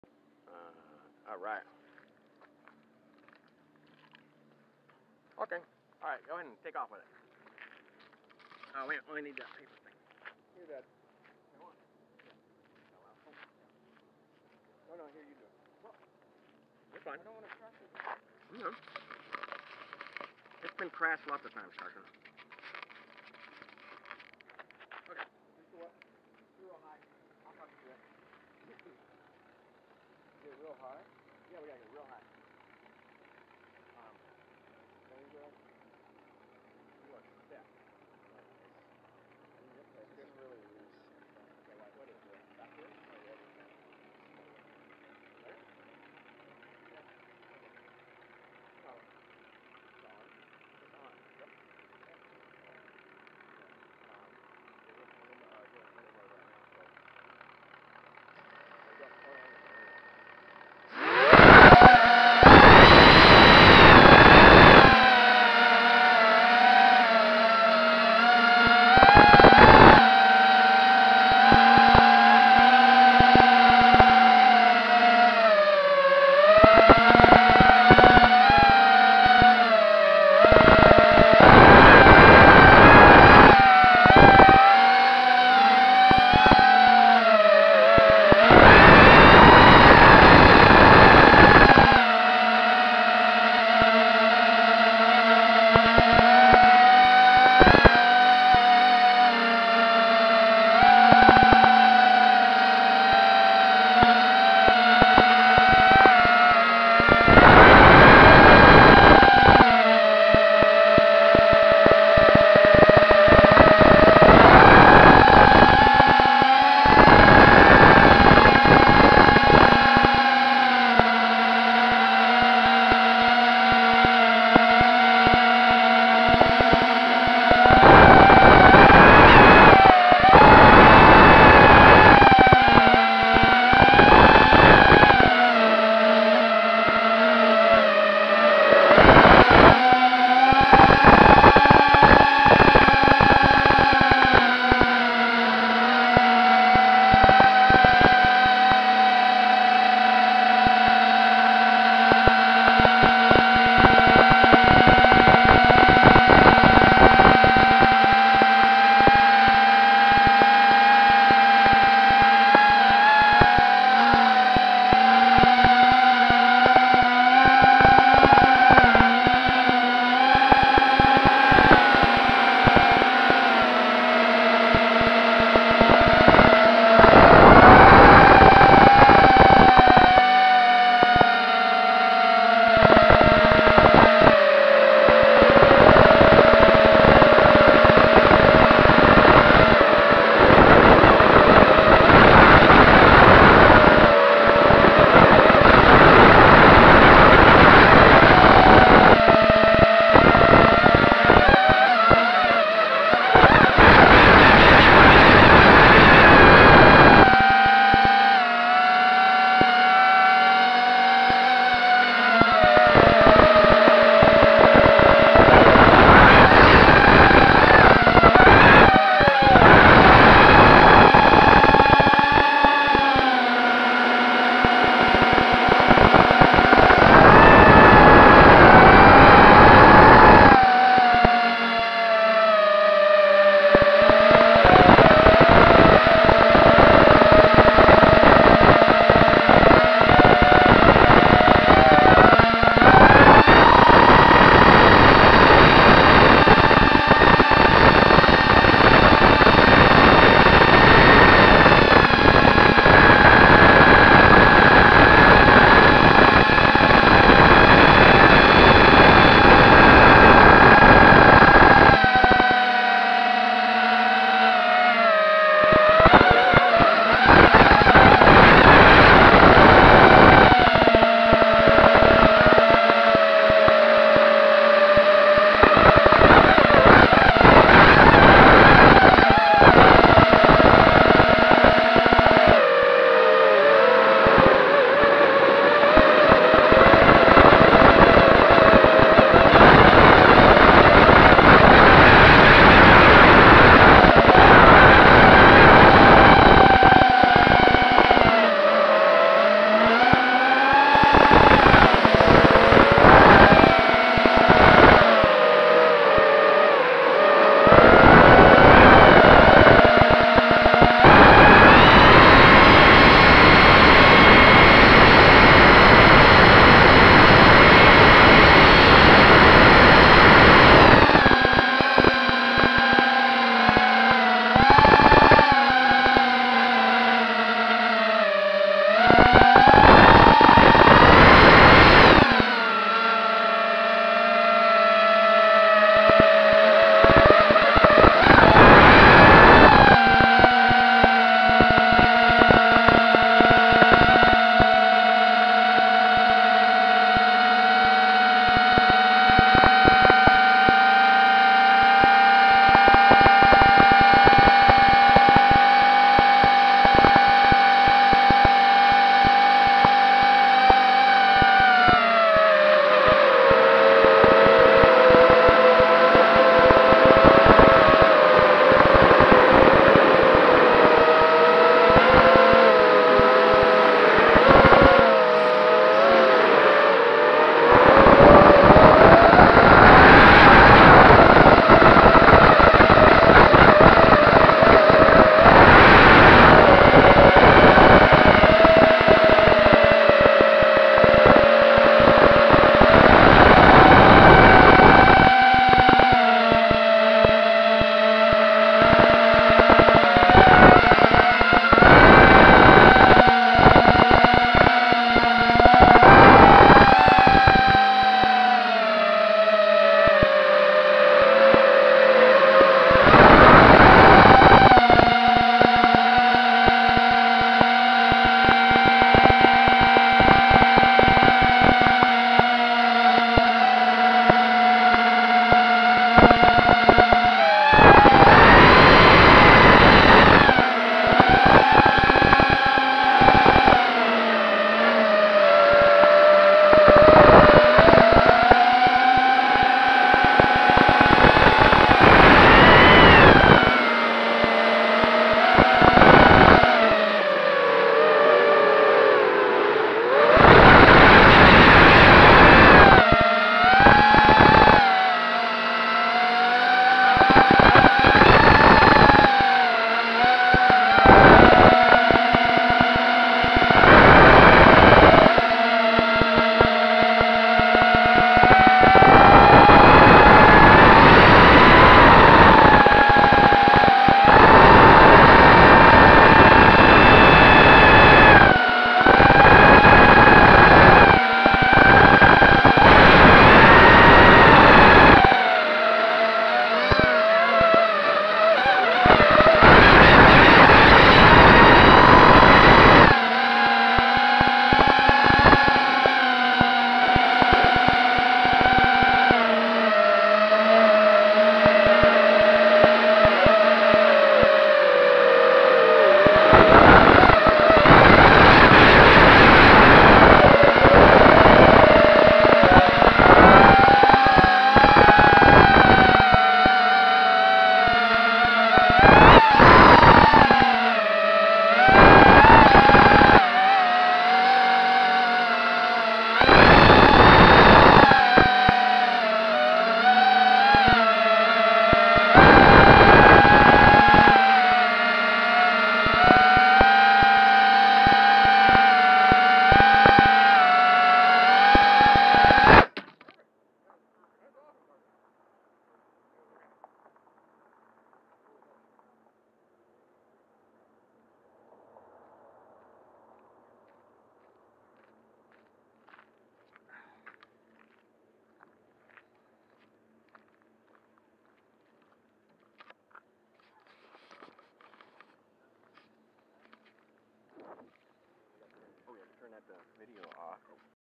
Aug 2019 - Arma, KS - Parade
General footage of Arma parade 2019 and other misc things.